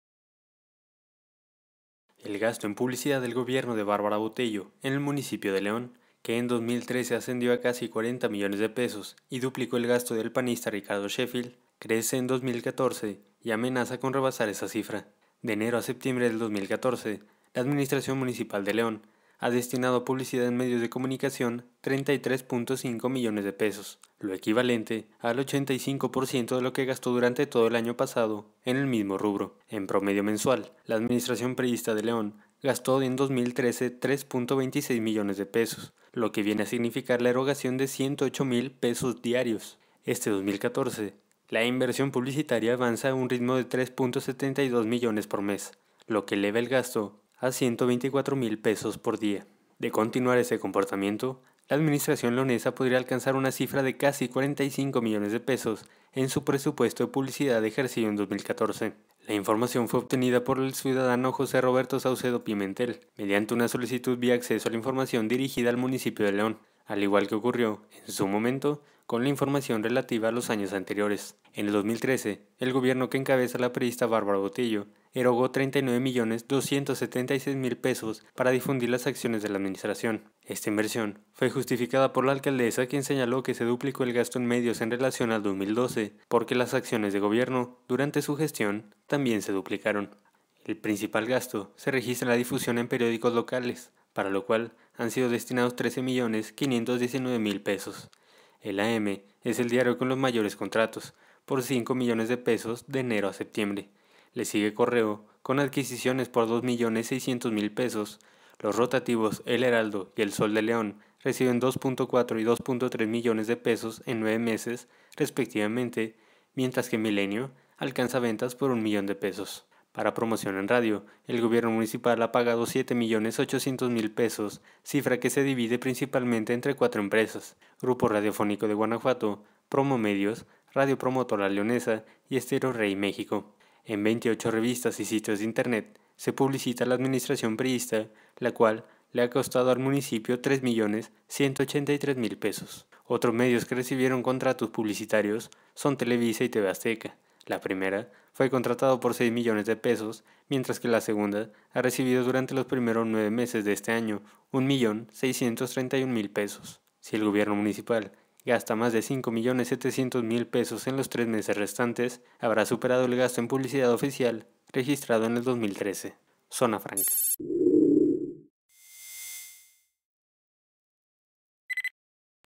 Crece ritmo de gasto publicitario de la administración Botello: gasta 124 mil pesos diarios en 2014.
León, Gto. El gasto en publicidad del gobierno de Bárbara Botello en el municipio de León, que en 2013 ascendió a casi 40 millones de pesos y duplicó el gasto del panista Ricardo Sheffield, crece en 2014 y amenaza con rebasar esa cifra.